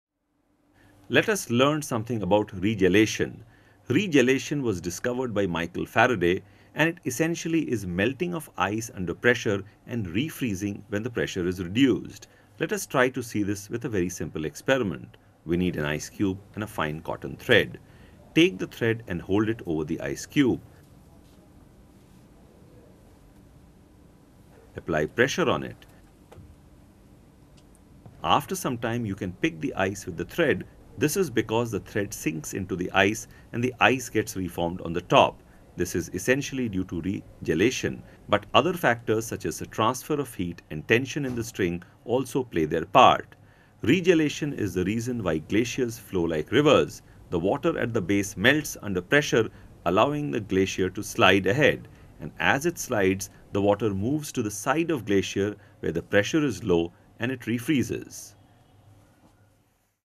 Length of string sinks into ice due to regelation | Pressure | Physics
Regelation is the increase of the melting point of a substance under pressure. A fascinating demonstration of this is possible by pushing a piece of string down on a block of ice. At the point of contact the pressure increases the melting point, causing the ice to melt, and letting the string sink. As soon as the pressure is past, the water again turns to ice, trapping the string inside. Glaciers move like rivers due to the property of regelation.